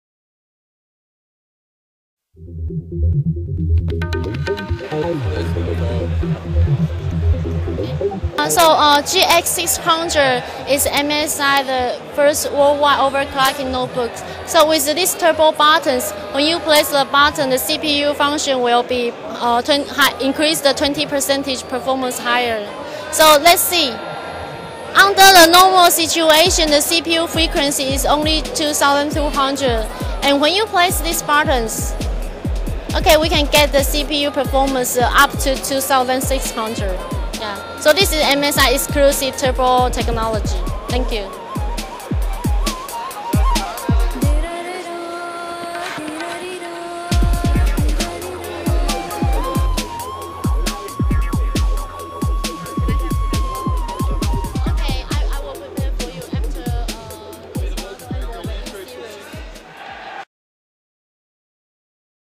MSI unveiled its 15.4-inch gaming notebook 'GX-600'
MSI unveiled its new 15.4-inch gaming laptop 'GX-600' during CeBIT 2008, which adopted 'Turbo Button' increasing the CPU speed up to 20%.